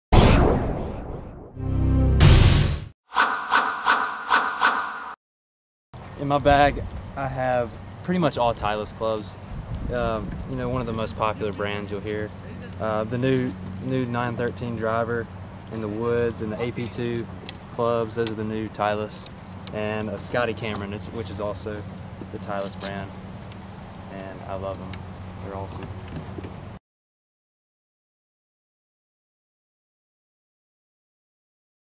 VIDEO: What's In Your Bag? Hayden Buckley
Mizzou Men's Golf player, Hayden Buckley, discussing the clubs in his bag. This is a production of the Mizzou Network.